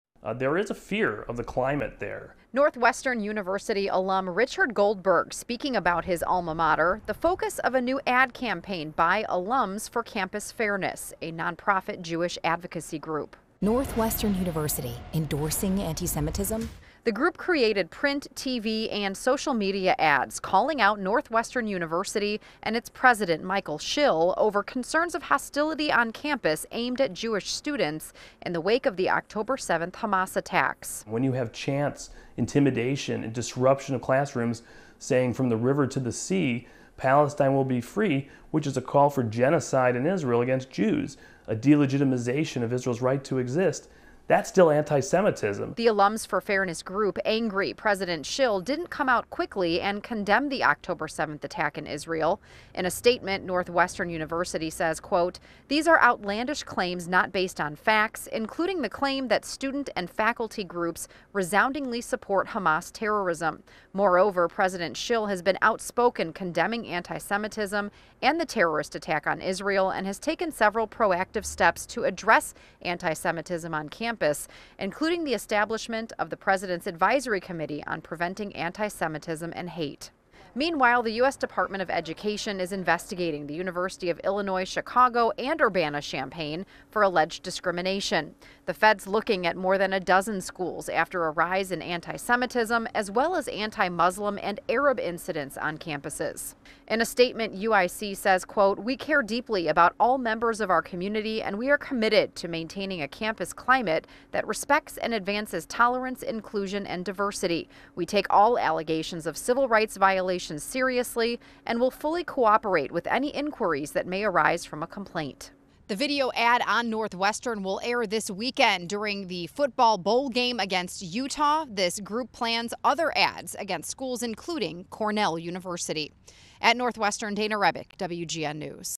Ad campaign calls out Northwestern University over antisemitism concerns
A Jewish advocacy group will air an advertisement during this weekend’s Northwestern-Utah bowl game following claims that Northwestern University has not been proactive about speaking out against antisemitism. WGN's Dana Rebik reports on the story.